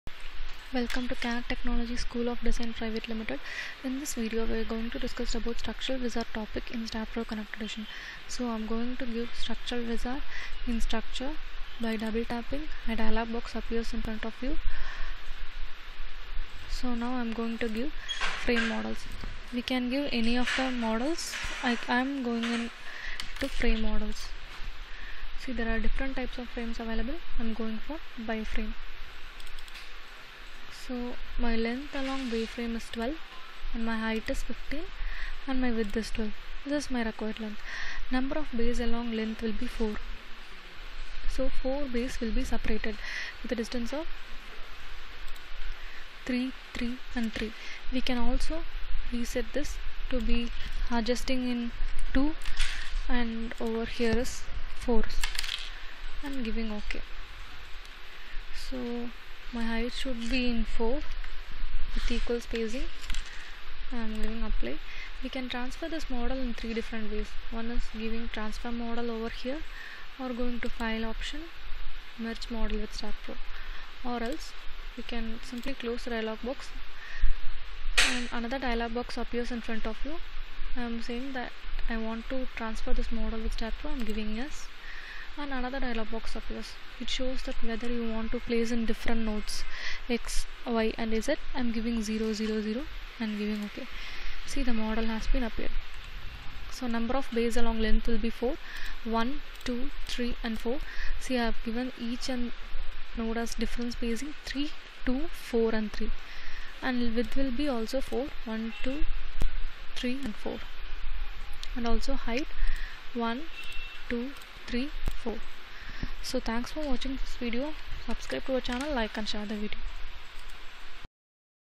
STRUCTURAL WIZARD - CREATION OF BAY FRAME MODEL,MERGE MODEL INTO STAAD PRO CONNECT EDITION V22
In this video, tutorial on structural wizard frame model is created in Staadpro connect edition V22.The video with real time examples are explained.